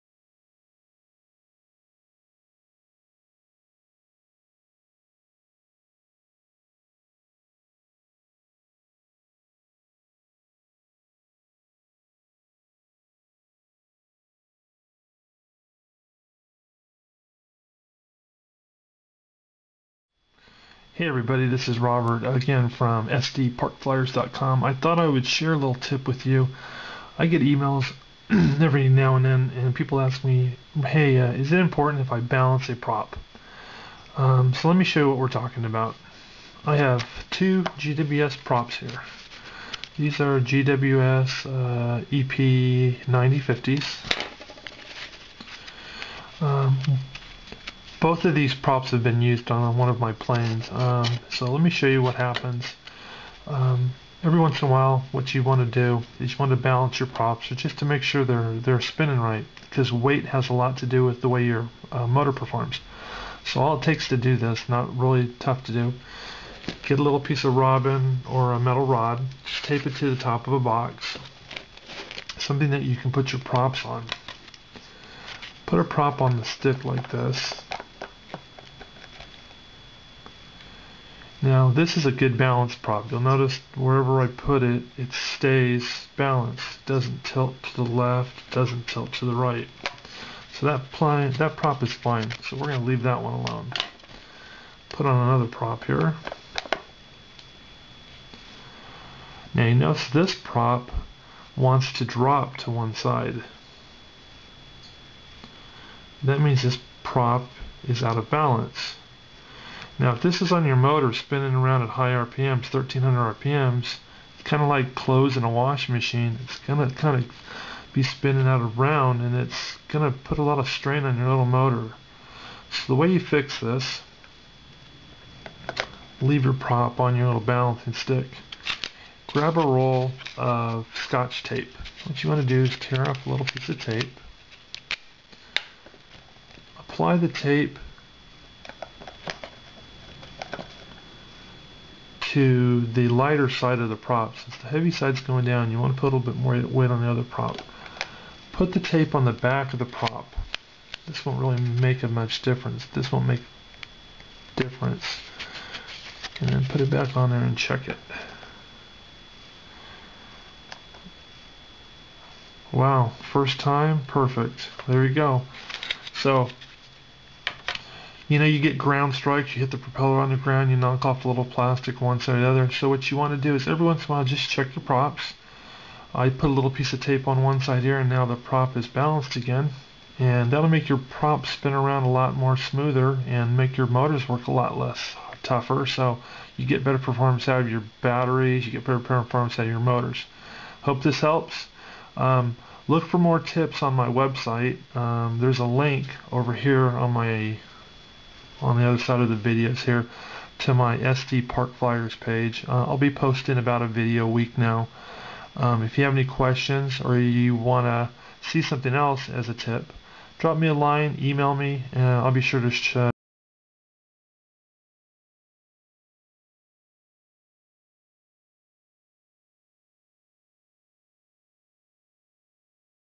How to balance a plastic Prop
Im also using the Dubro Prop Balancer in the New Video.

How to balance a plastic propeller using some coming items and scotch clear tape
"Not saying this is the Best way to balance a prop but a simple quick method that has been used to fix many props"

Here is another Video I did that shows the vibration that
can be caused by a unbalanced propeller